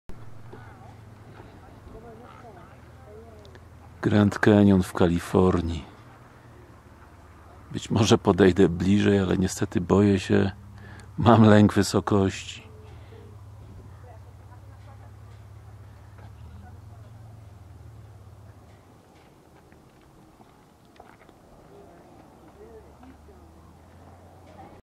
Grand Canyon Wielki Kanion w 2012 roku
Grand Canyon Wielki Kanion w 2012 roku. Kolejny z filmików z mojego pobytu w Stanach Zjednoczonych. Widoki naprawdę fantastyczne i dosłownie zapierające dech w piersiach. Proszę popatrzeć na te atrakcje turystyczne samemu.